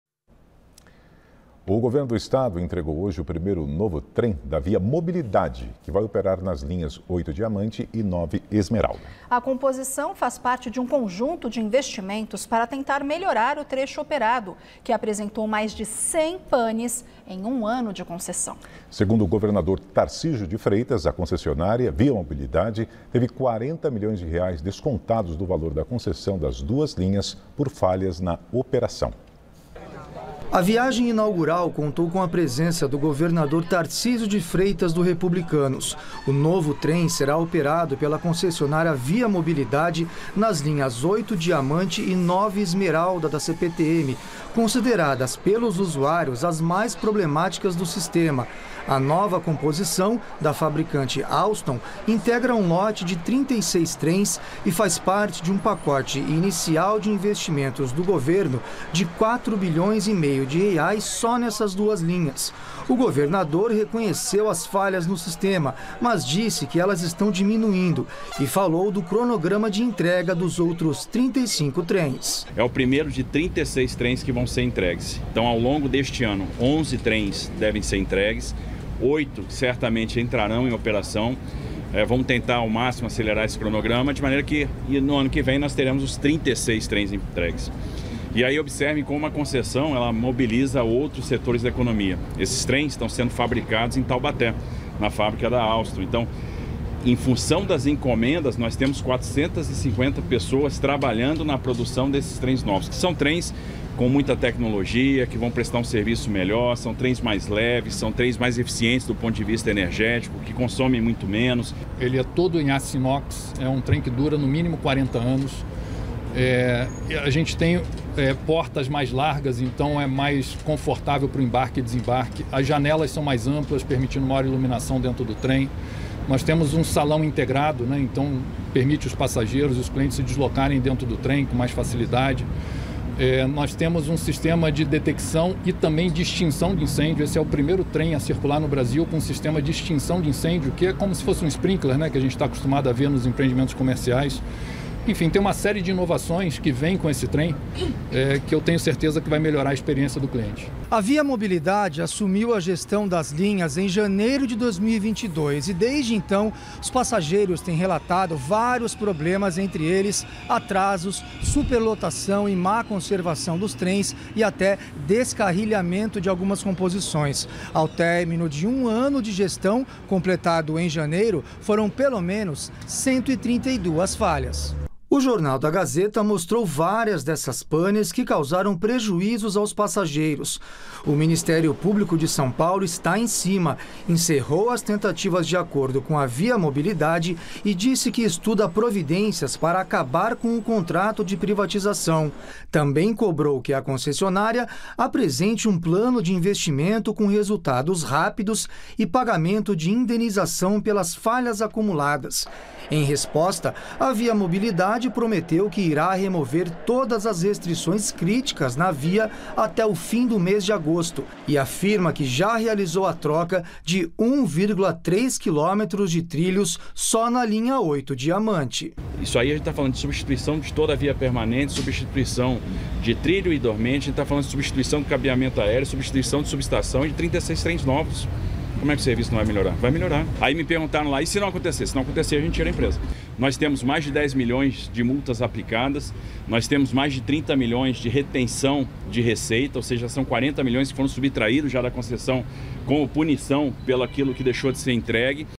ViaMobilidade teve R$ 40 milhões descontados no valor da concessão das linhas 8 e 9
O Governo do Estado entregou hoje o primeiro novo trem da ViaMobilidade, que vai operar nas linhas 8-Diamante e 9-Esmeralda. A composição faz parte de um conjunto de investimentos para tentar melhorar o trecho operado, que apresentou mais de cem painéis em um ano de concessão. Segundo o governador Tarcísio de Freitas, o empresário ViaMobilidade teve 40 milhões de reais descontados do valor da concessão das duas linhas, por falhas na operação.

Foto: Rovena Rosa/Agência Brasil